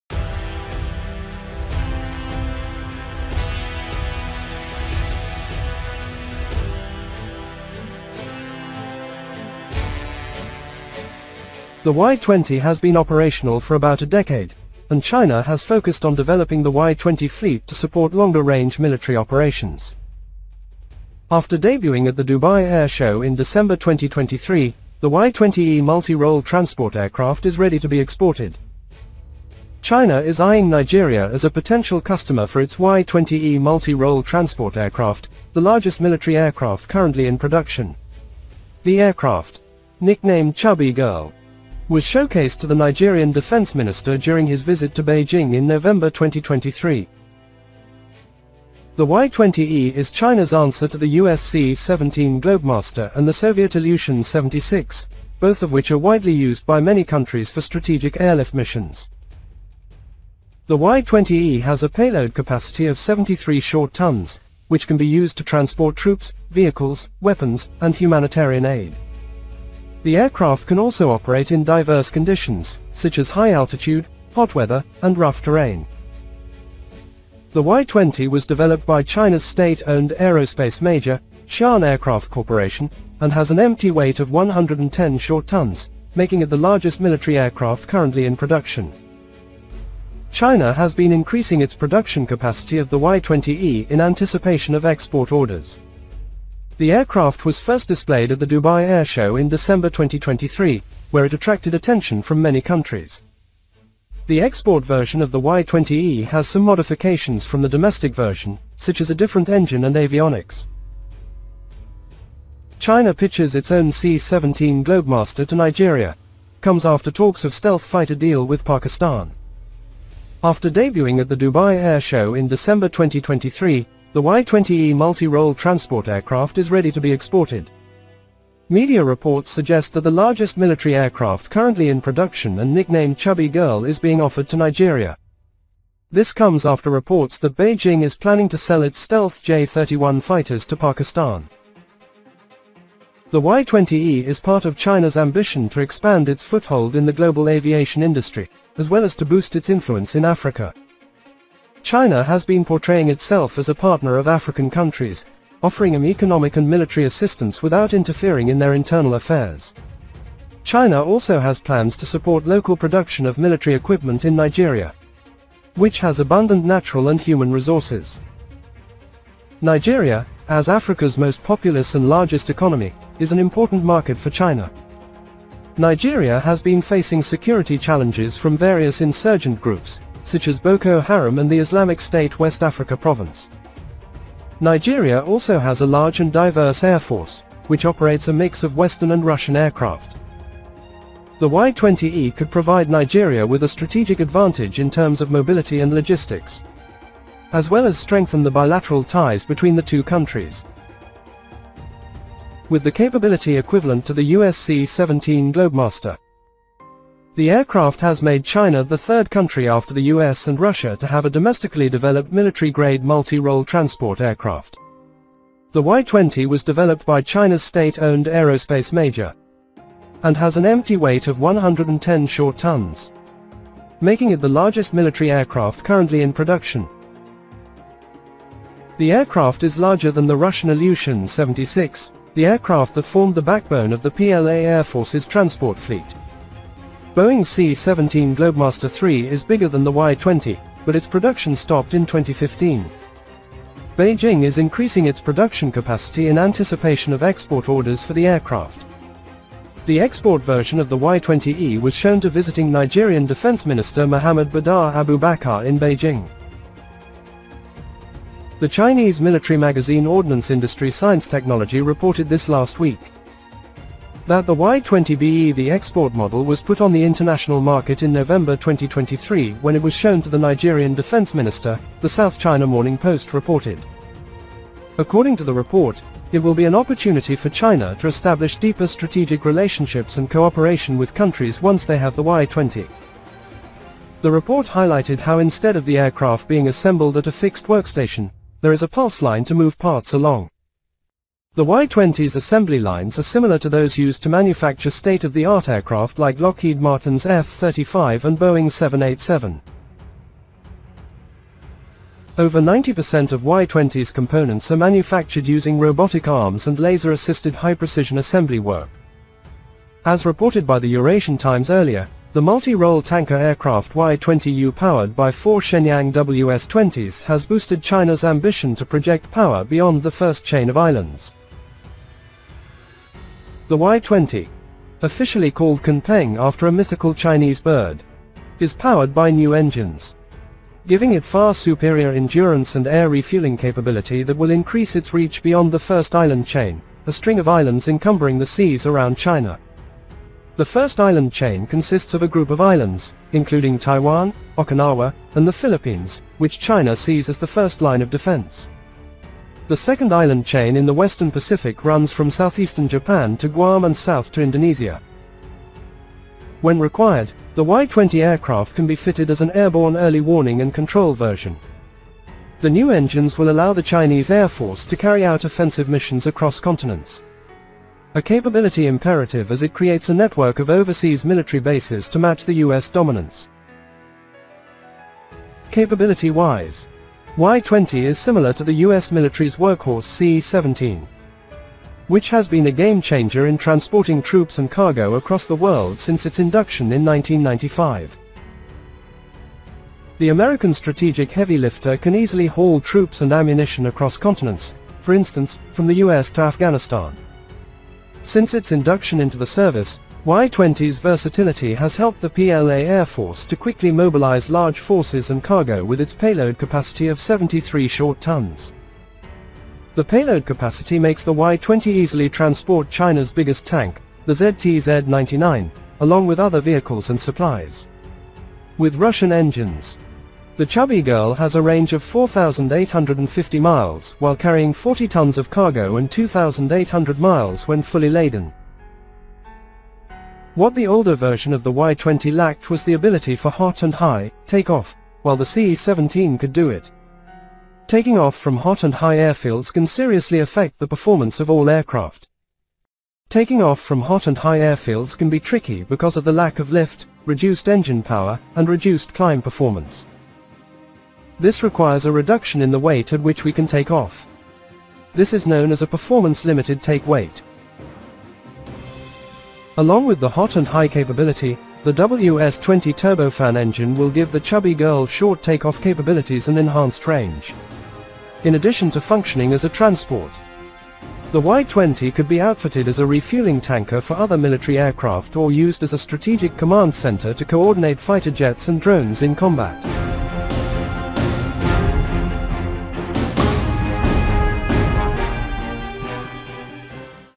Y-20 The Largest Military Aircraft to Nigeria, is China’s answer to the US C-17 Globemaster
China markets its Y-20 strategic airlift aircraft to Nigeria, Y-20 is the largest aircraft in the China's Air Force. The Y-20E multi-role transport aircraft is ready to be exported. China is eyeing Nigeria as a potential customer for its Y-20E multi-role transport aircraft, The Y-20E is China’s answer to the US C-17 Globemaster and the Soviet Ilyushin 76, both of which are widely used by many countries for strategic airlift missions. The Y-20 E has a payload capacity of 73 short tons, which can be used to transport troops, vehicles, weapons, and humanitarian aid. The aircraft can also operate in diverse conditions, such as high altitude, hot weather, and rough terrain. The Y-20 was developed by China’s state owned aerospace major, Xian Aircraft Corporation, and has an empty weight of 110 short tons, making it the largest military aircraft currently in production.